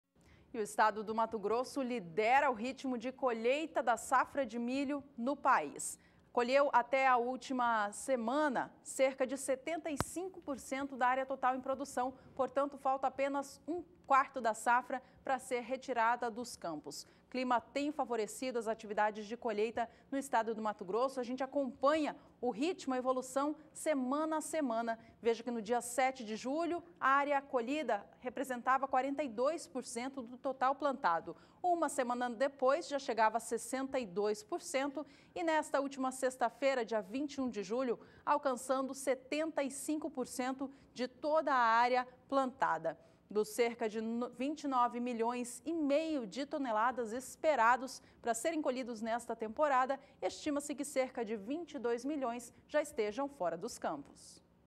NOTA MILHO MT - 24/07/2017
Colheita de milho no MT chega a 75% dos quase 30 mi/ton esperados

A colheita do milho de segunda safra no Mato Grosso já está no quarto final. Alcançou até o dia 21 de julho 75% da área total plantada. Com isso, estima-se que o estado já colheu mais de 22 milhões de toneladas dos 29,5 milhões esperados para esta safra.